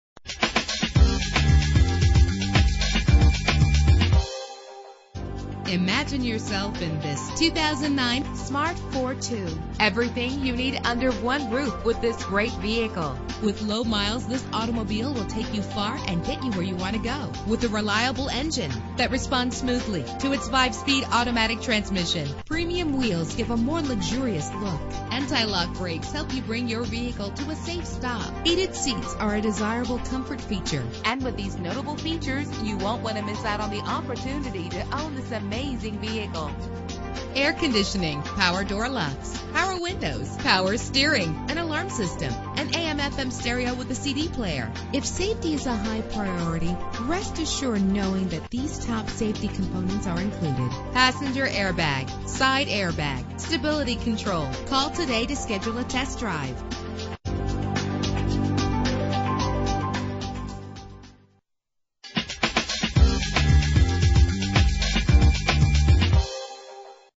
2009 smart fortwo - Manassas Va
Year: 2009
Make: smart
Model: fortwo
Trim: BRABUS
Engine: 1.0 liter inline 3 cylinder 12 valve
Transmission: 5-Speed Automatic
Color: Silver Metallic
Mileage: 19097
Address:
9010 Liberia Avenue